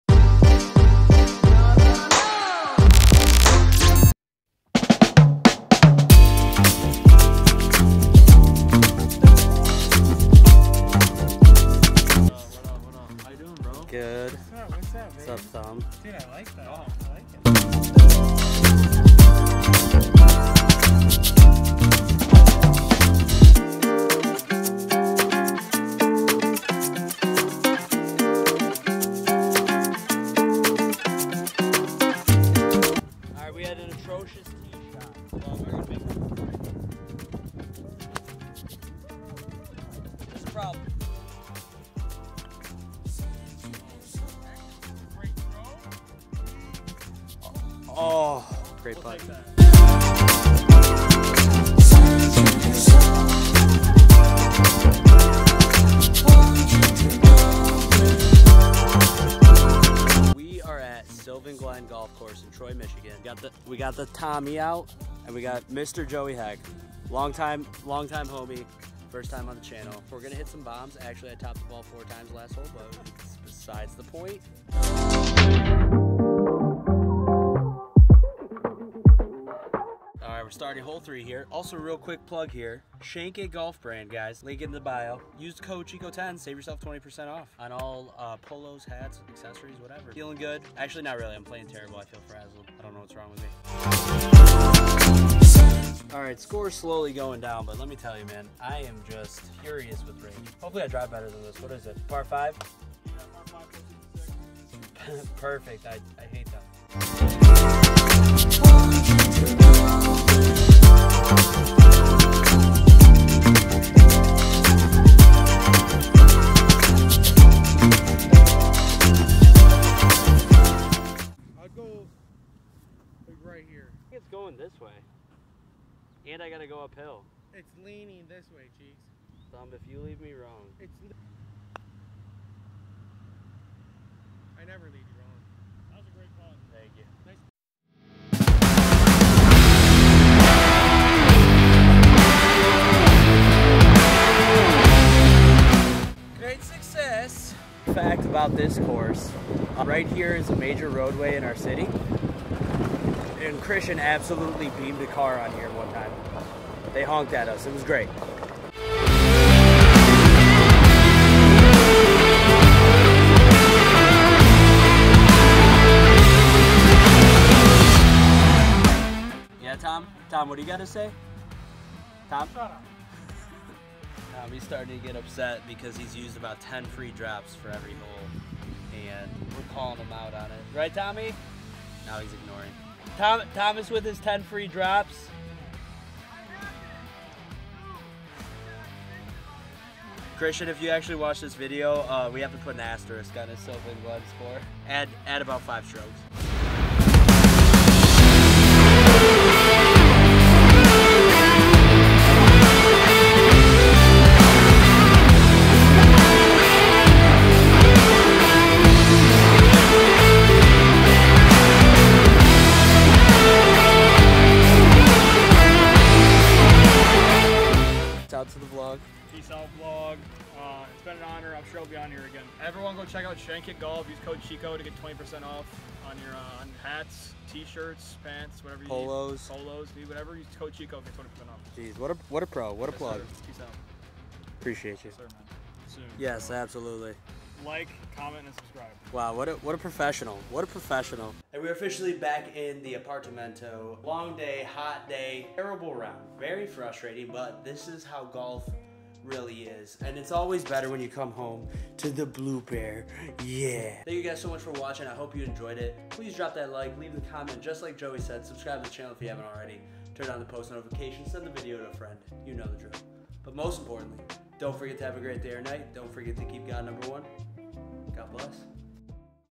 golfing at 100+ year old course | Sylvan Glen Golf Course
in today's video - we are back with more golf!! is this becoming a golf channel? lol anyway, Tommy and the long time homie, Joey, and I went to Sylvan Glen golf course which is a super nice course in our area. it is over 100 years old and plays really nicely! our league play is definitely intensifying but overall, it's proving to be a great year of golf! let me know down below if you have ever played Sylvan Glen and what you think if you have! thank you for watching - i hope you enjoyed the video - God Bless :D